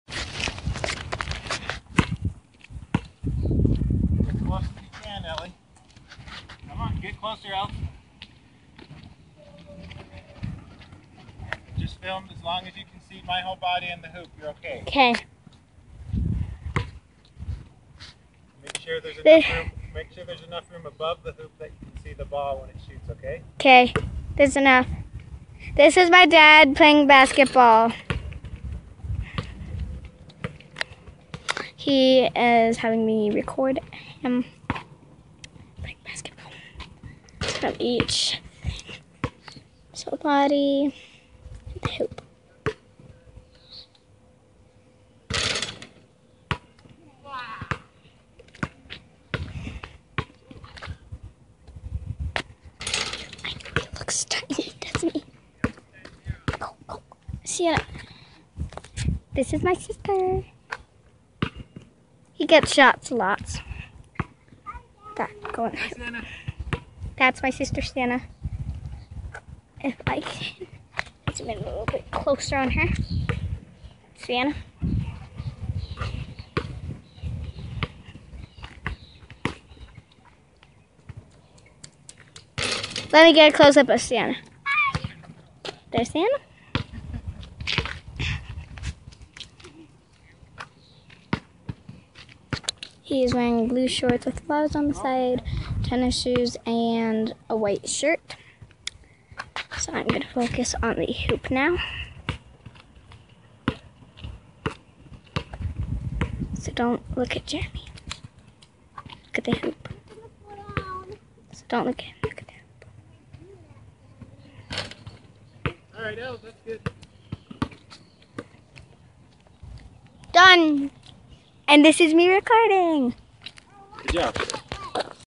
Playing basketball with Ellie filming and Sienna playing
This is me (Jeremy) playing basketball while my daughter Ellie films with my phone and Sienna plays around :)